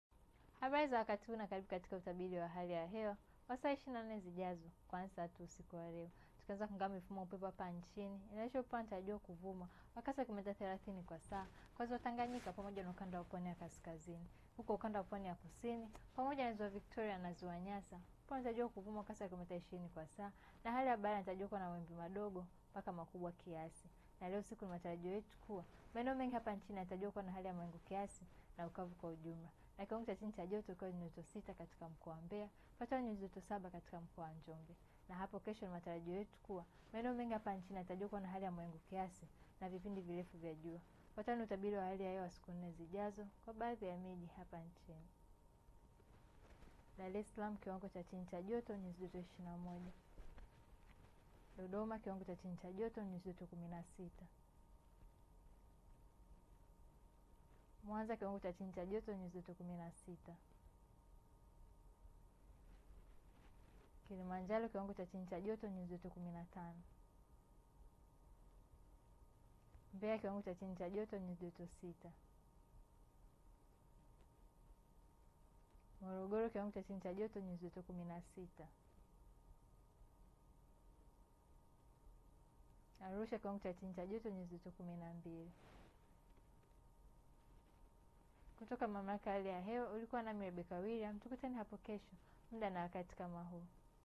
UTABIRI WA HALI YA HEWA 27/07/2024
Utabiri wa hali ya hewa wa saa 24 zijazo na Rebecca Luhitana.